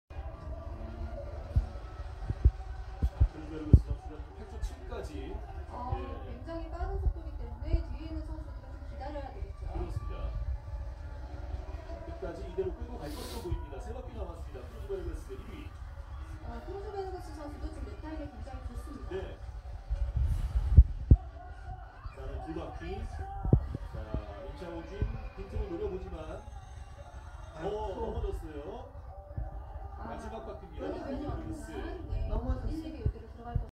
Lin Xiajun (formerly known as 임효준 Lim hyo jun) Slips away 2023
22-23 ISU short track world cup dresden quarterfinals